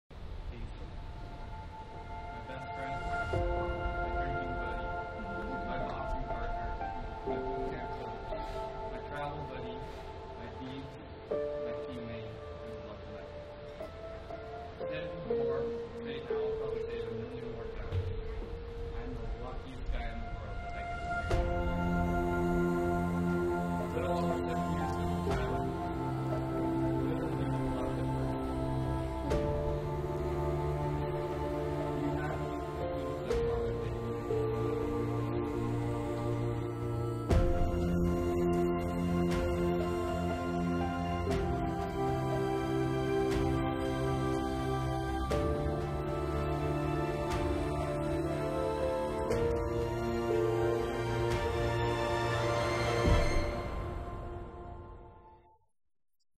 Wedding Teaser - Best Ever vows
The most beautiful words we can hear. Every word comes from the bottom of the heart, this is Love.

Wedding at Secrets the Vine Cancun yesterday afternoon. Simply beautiful.